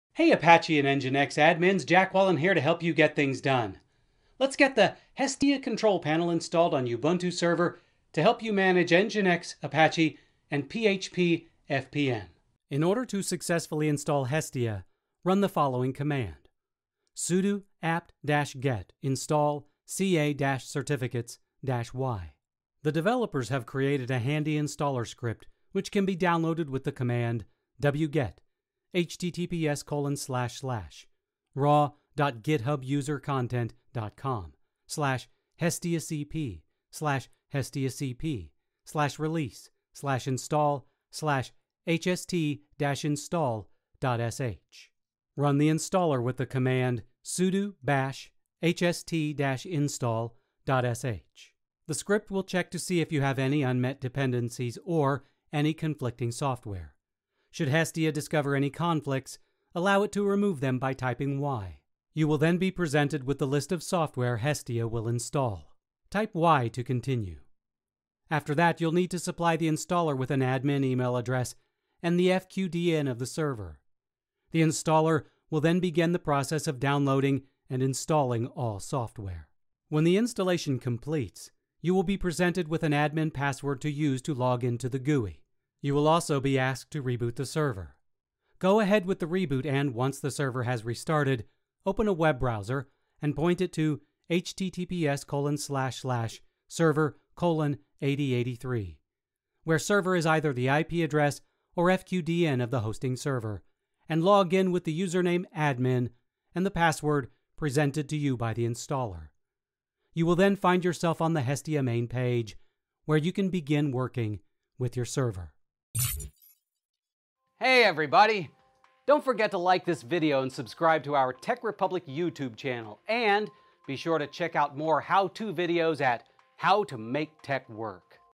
How to install the Hestia Control Panel
Hestia is a web-based GUI for configuring NGINX, Apache, and PHP-FPM. Jack Wallen shows you how to get this up and running on Ubuntu Server 20.04.

EDITOR'S NOTE: This video was originally published January 2021 on TechRepublic. This information may be outdated depending on time of viewing.